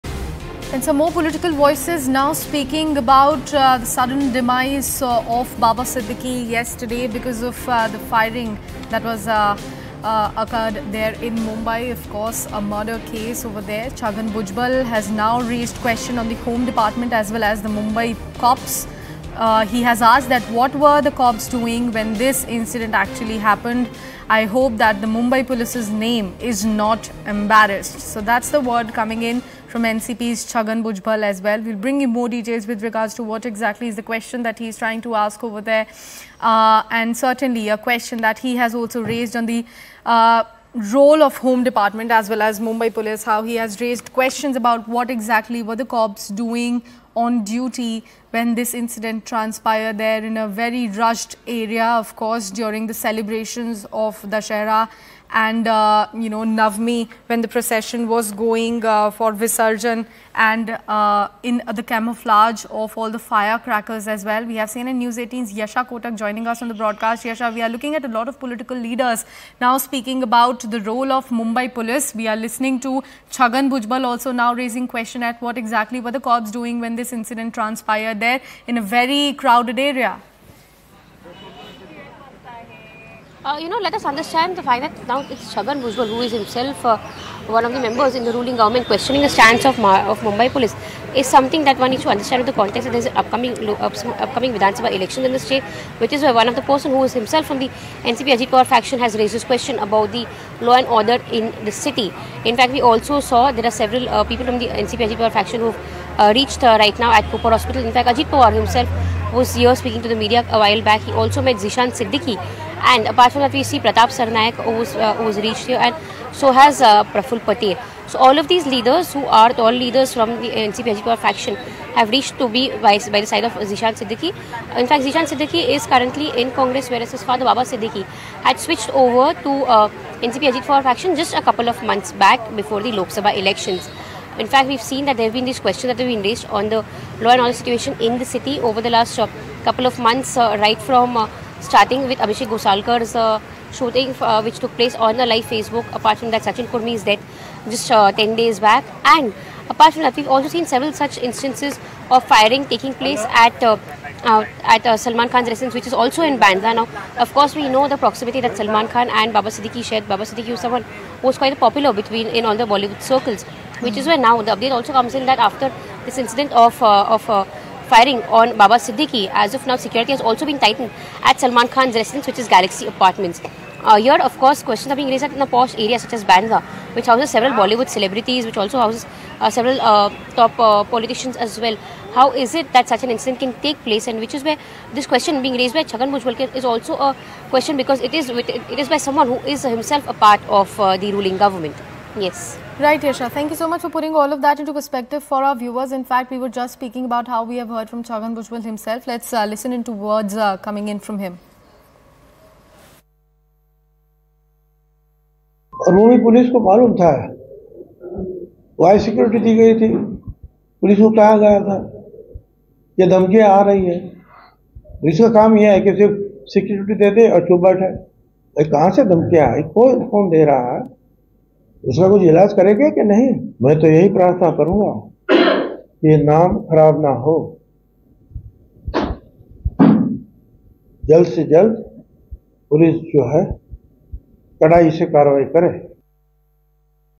Chhagan Bhujbal Lashes Out At Home Dept. And Mumbai Police On Baba Siddique Murder | News18
Chhagan Bhujbal lashes out at Home Dept. and Mumbai Police on Baba Siddique murder